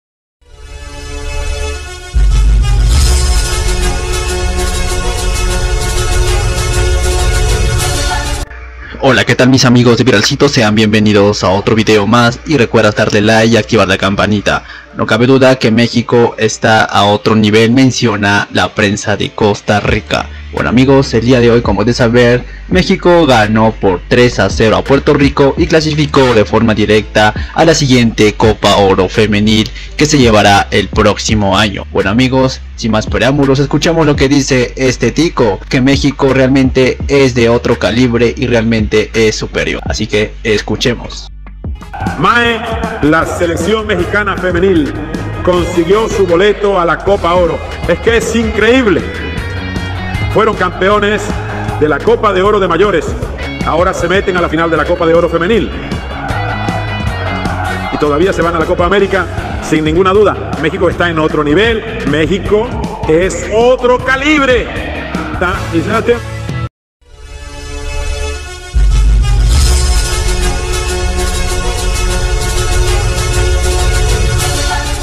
TICOS SE RINDEN ANTE MEXICO ESTAN EN TODOS LOS TORNEOS SON DE OTRO CALIBRE Y NIVEL 3-0 QUE ENVID1A
PRENSA DE COSTA RICA  RINDEN ANTE MEXICO ESTAN EN TODOS LOS TORNEOS SON DE OTRO CALIBRE Y NIVEL